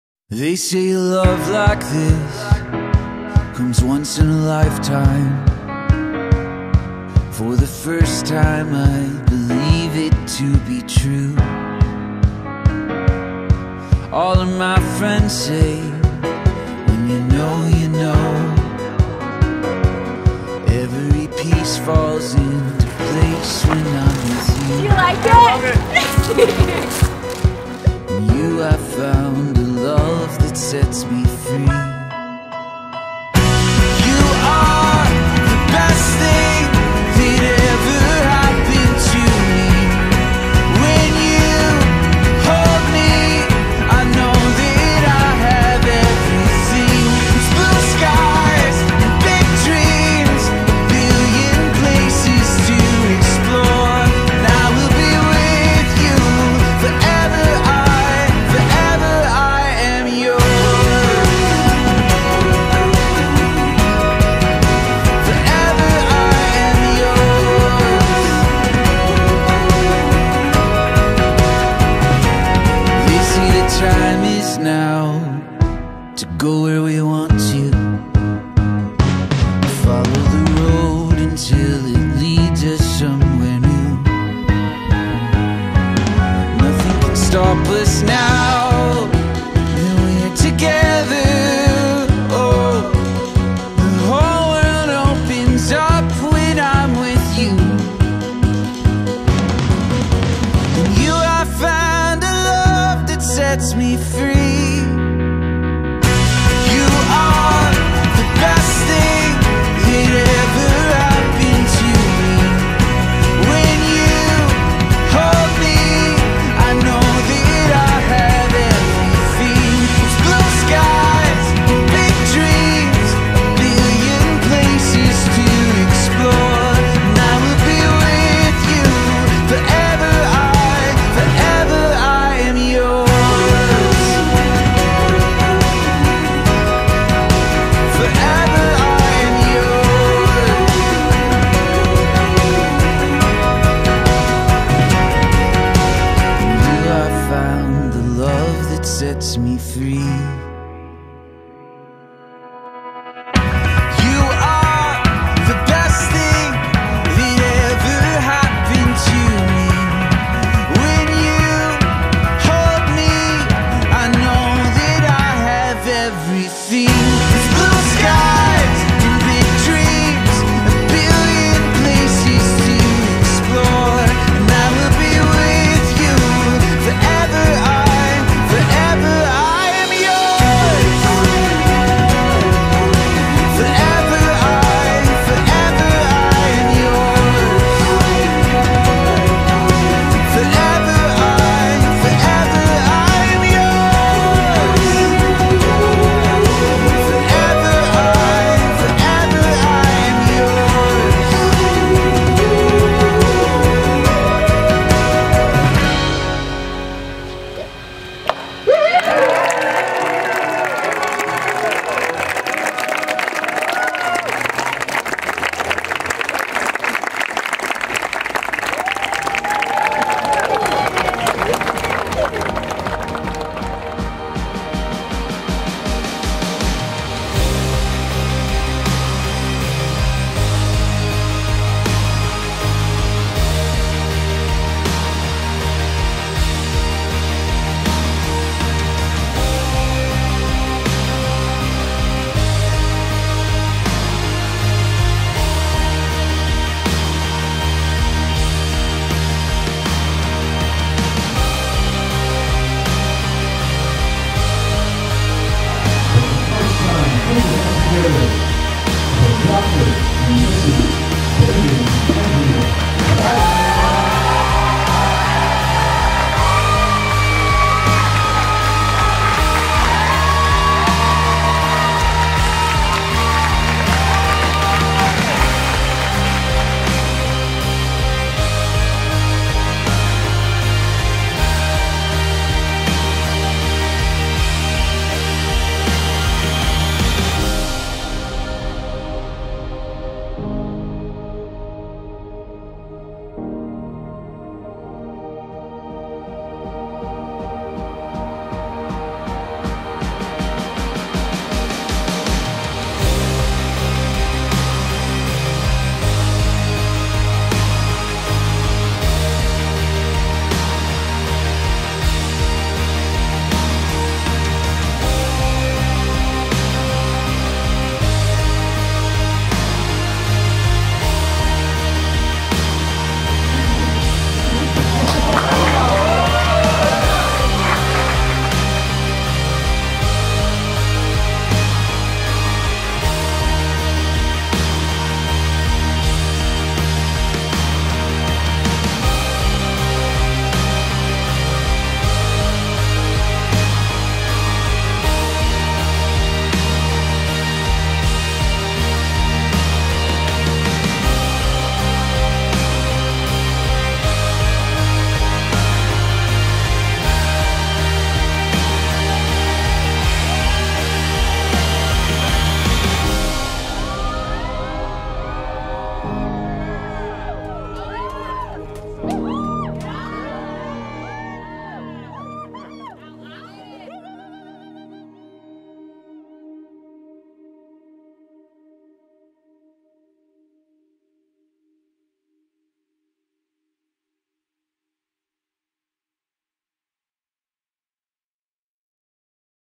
Umlauf Sculpture Garden and Museum Wedding Video by DanielKaVideo | Austin Wedding Videographer
Highlight film of Andrea and David's wedding at Umlauf Sculpture Garden and Museum in Austin, TX on Friday November 02, 2018.
Camera and editing: DanielKaVideo.